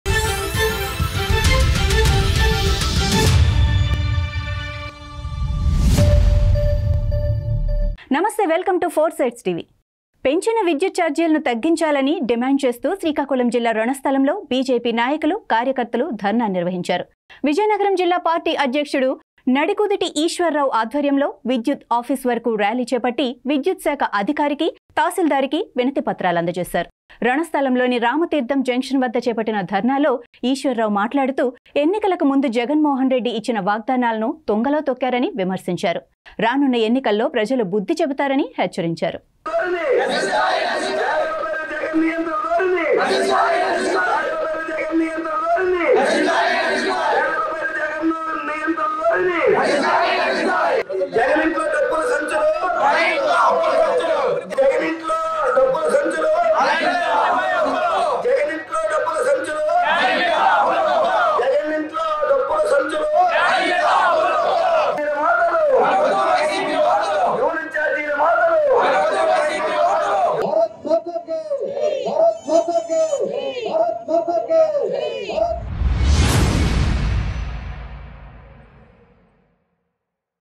సీఎం జగన్ కు ప్రజలు బుద్ధి చెబుతారని హెచ్చరిక | BJP dharna on electricity charges | 4Sides Tv Telugu
AP News : BJP leaders and activists staged a dharna at Ranasthalam in Srikakulam district demanding a reduction in the increased electricity charges. Under the leadership of Vizianagaram District Party President Nadukuduthi Eswara Rao, a rally was taken up to the electricity office and petitions were handed over to the electricity department officer and tehsildar. Speaking at the dharna at Ramtheertham junction in Ranasthalam, Iswara Rao criticized Jagan Mohan Reddy for breaking his promises before the elections. He warned that people will be wise in the upcoming elections.